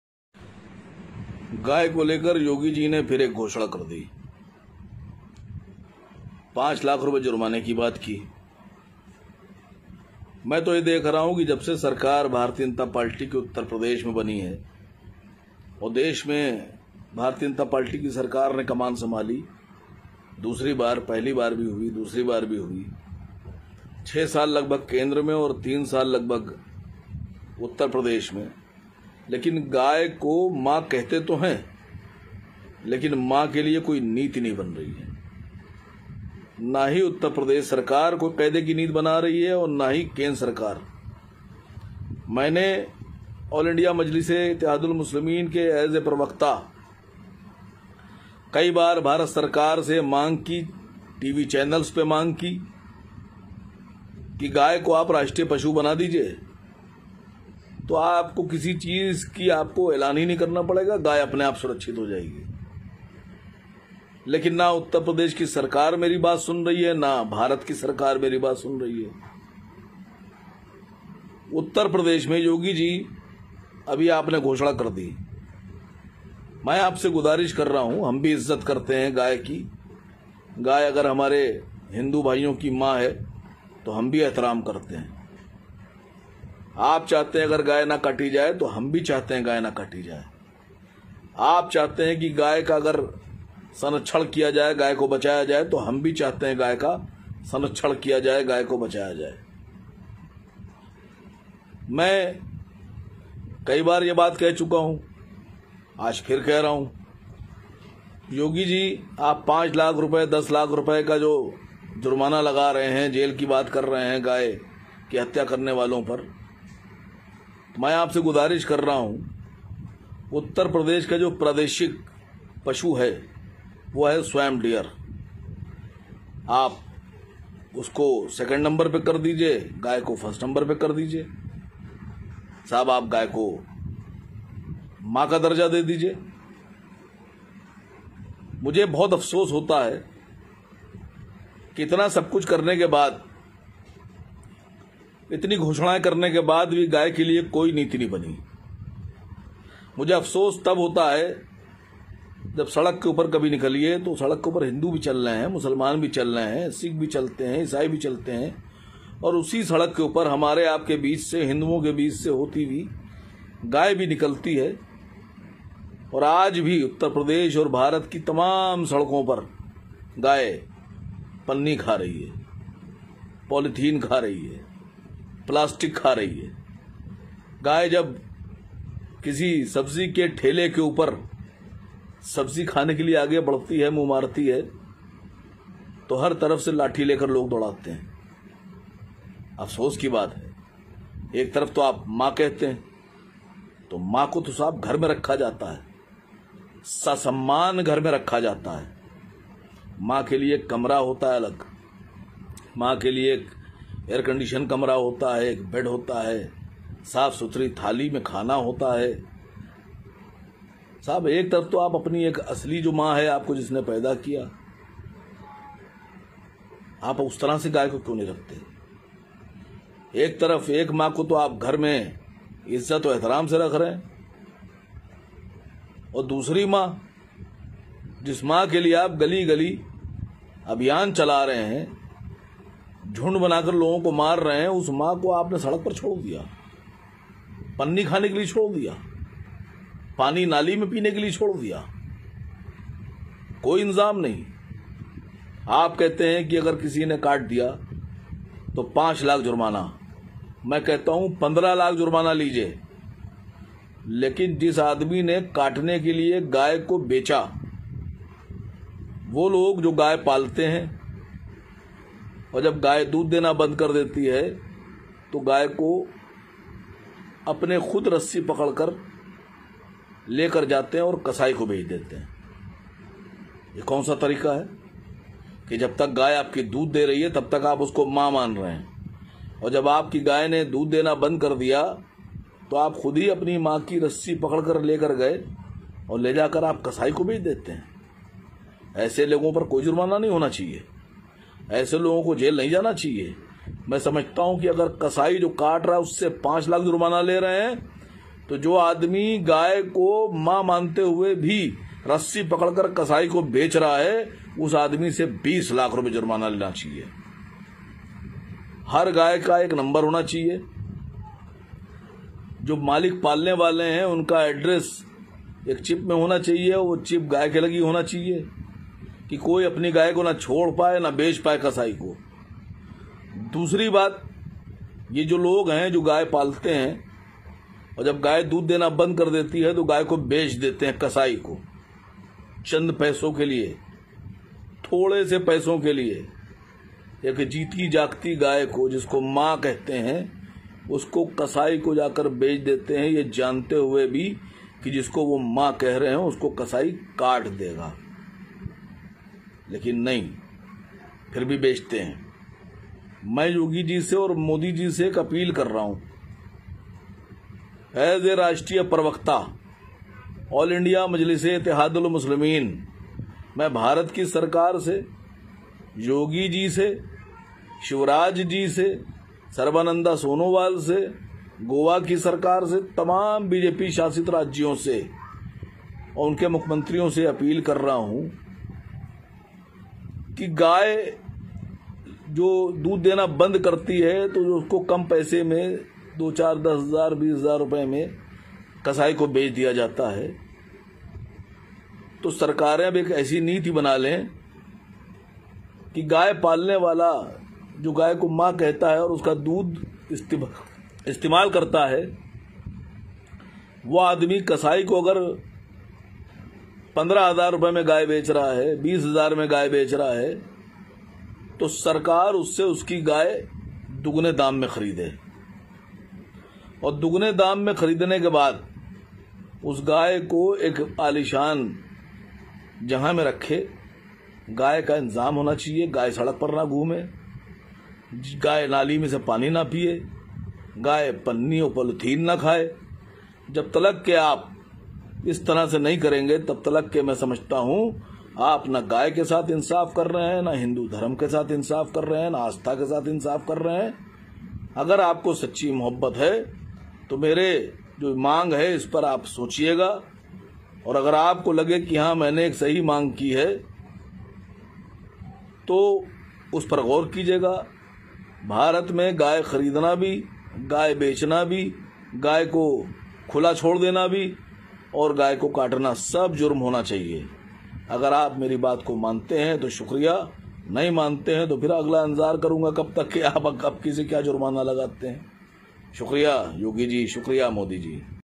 नरेंद्र मोदी से बोला ओवैसी का राष्ट्रीय नेता 'गाय को दो माँ का दर्जा' ! गौमाता राष्ट्रमाता घोषित हो
योगी जी के उत्तर प्रदेश में गौरक्षा के कड़े कानून पर 5 लाख जुर्माने और 10 वर्ष की सजा पर सईद असीम वकार ने बहुत गंभीर बात कही है जिसे कहने की हिम्मत अच्छे अच्छे नही कर सकते।

मोदी जी और योगी जी को AIMIM के राष्ट्रीय प्रवक्ता सईद असीम वकार ने बहुत गंभीर 2 टूक बात कही है जिसे कहने की हिम्मत अच्छे अच्छे पार्टी, हिन्दू नेता नही कर सकते वो देशद्रोही ओवैसी की पार्टी का राष्ट्रीय प्रवक्ता पूरी जिम्मेदारी से कह रहा है।

🚩हर सनातनी के लिए गाय हमारी माता है, पशु नही इसलिए गौमाता को राष्ट्रमाता घोषित कर देना चाहिए।

आखिर किस कारण विलंब कर रही है केंद्र सरकार गौमाता को राष्ट्रमाता का केंद्रीय कानून बनाने में ?